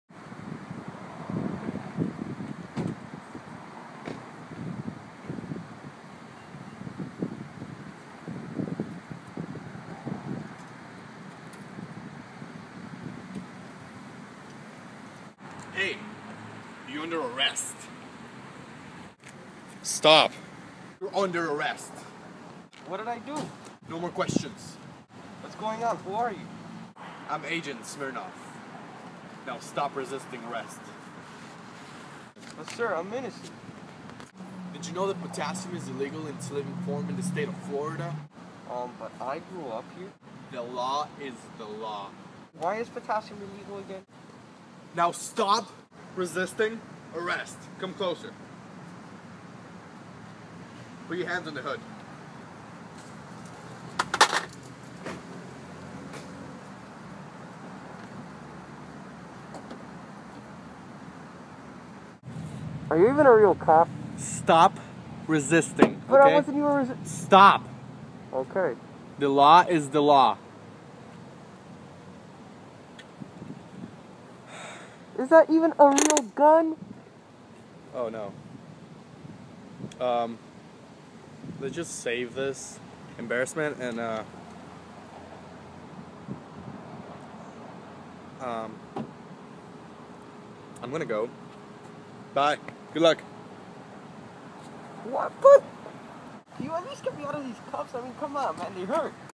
Banana GETS ARRESTED
Florida decided to make potassium illegal in it's living form
THUMBS UP AND FAVORITE!!

Starring
Danny Llerena as Banana
Alexander Korotkov as Smirnoff

Written by us